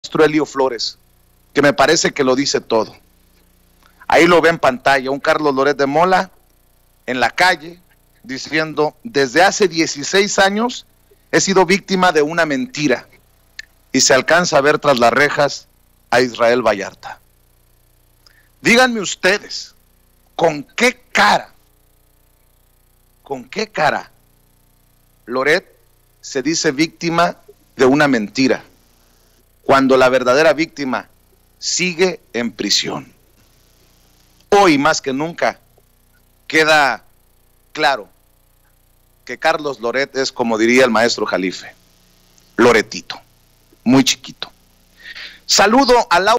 ¿CON QUÉ CARA LORET SE HACE LA VÍCTIMA, CUANDO LA VERDADERA VÍCTIMA SIGUE EN PRISIÓN?
El maestro Helio Flores muestra de manera magistral la realidad del pseudoperiodista Carlos Loret de Mola, mejor conocido como LordMontajes por precisamente la serie de montajes que ha realizado y aceptado, que mientras él se queja de un ataque contra su persona por 16 años, Israel Vallarta lleva 16 años en prisión.